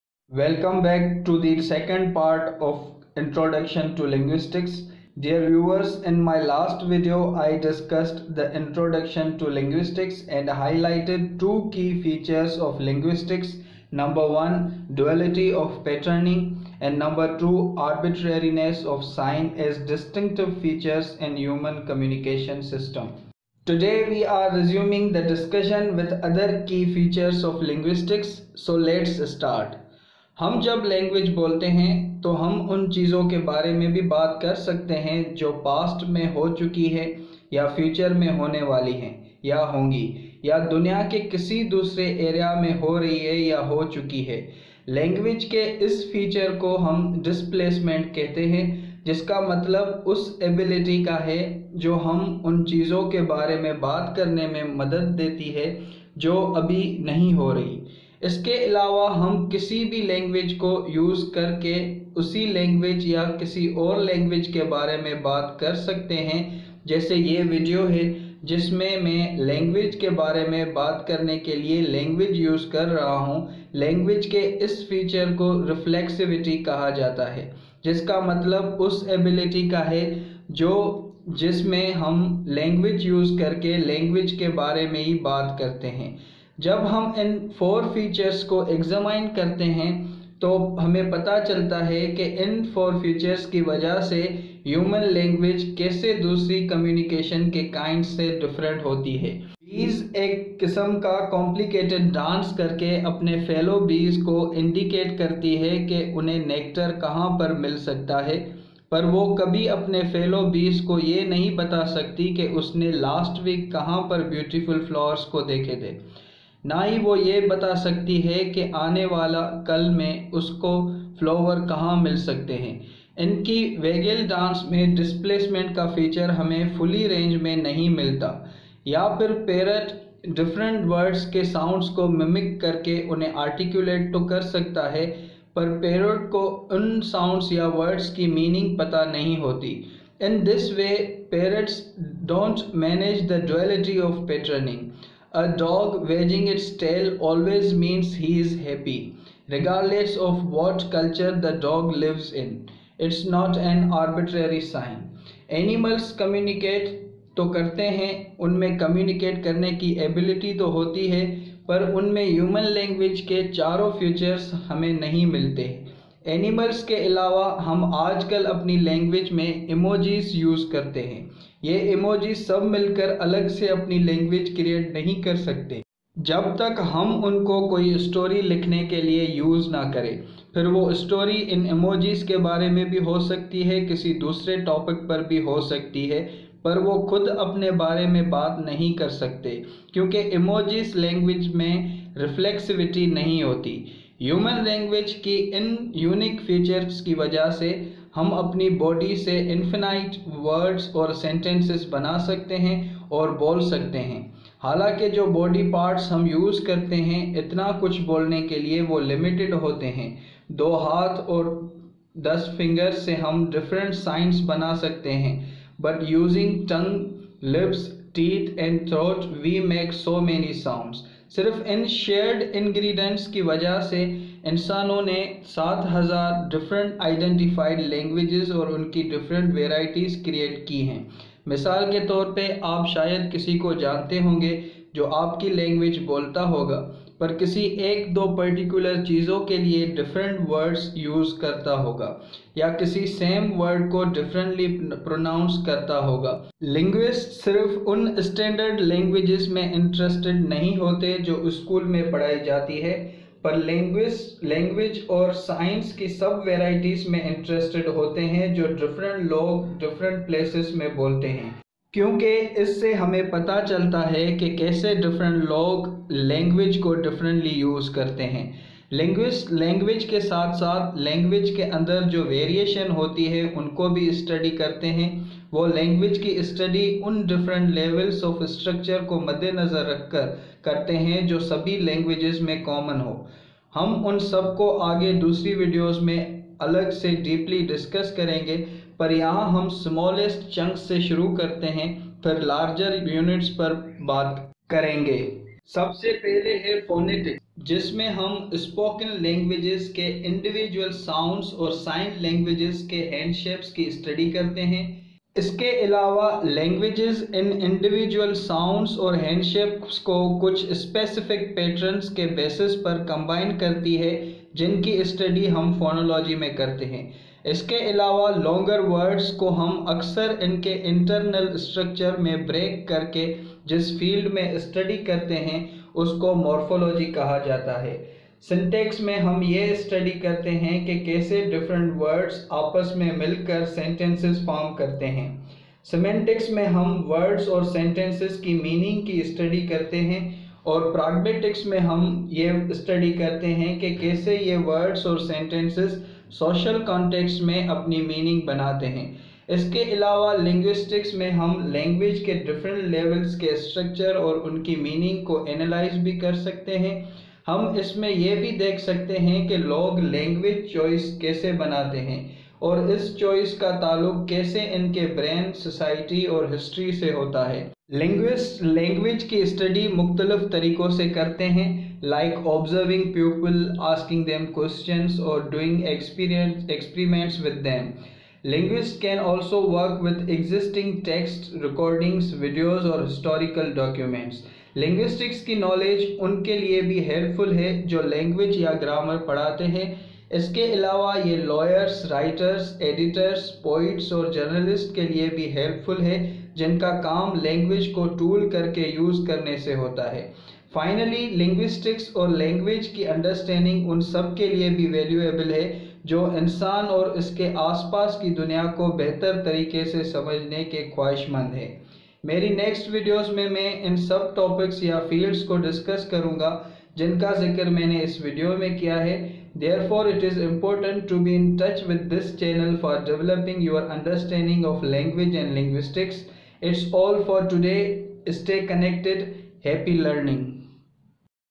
What is Linguistics? Part 2 in Hindi / Urdu
Linguistics is the study of language whether it is human language or any other form or system of communication.